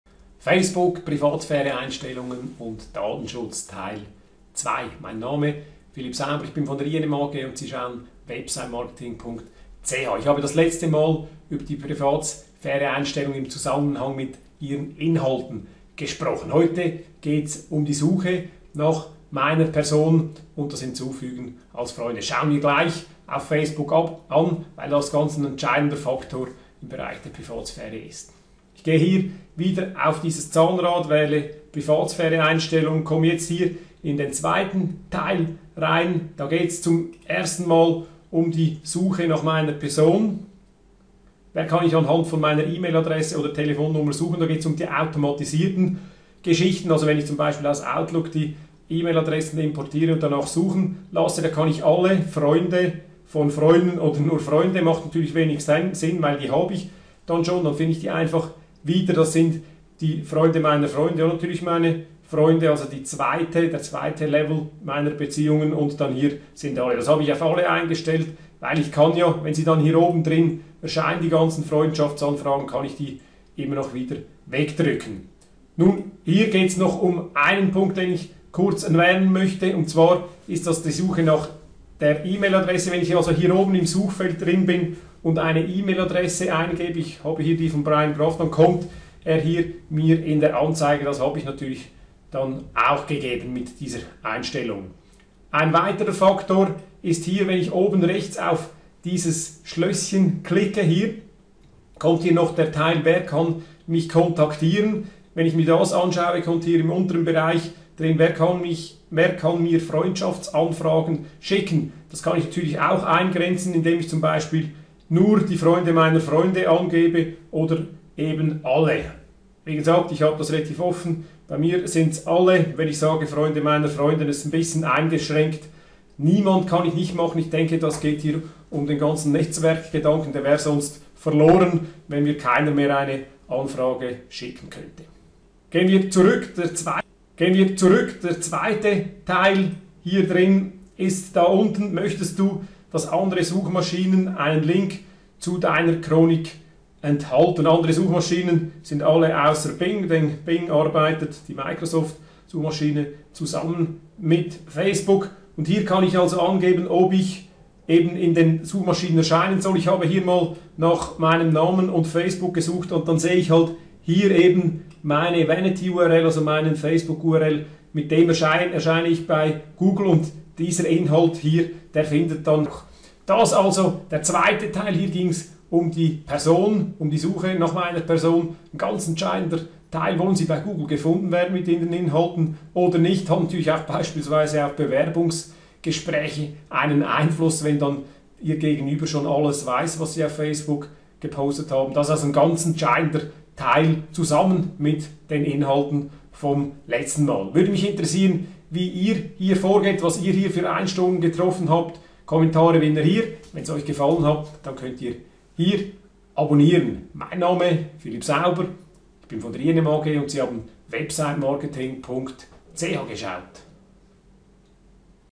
Facebook Privatsphäre Einstellungen und Datenschutz (Tutorial Teil 2)
- Die Facebook Privatsphäre Einstellungen und der Datenschutz sind ein heisses Thema. Welche meiner Inhalte auf Facebook werden wem gezeigt. Und wer kann nach mit Suchen bez. erscheint mein Facebook Profil auf Google und Co. Das sind entscheidende Fragen rund um Facebook.
Im Video wird Schritt für Schritt gezeigt, wie man die Einstellungen bei den Facebook Privatsphäre Einstellungen und dem Datenschutz richtig setzt, sodass eben nur das angezeigt wird was auch öffentlich sein soll.

Auf Feedback und Verbesserungsvorschläge freue ich mich wie immer!

Danke und schöne Grüsse
Philipp Sauber
INM AG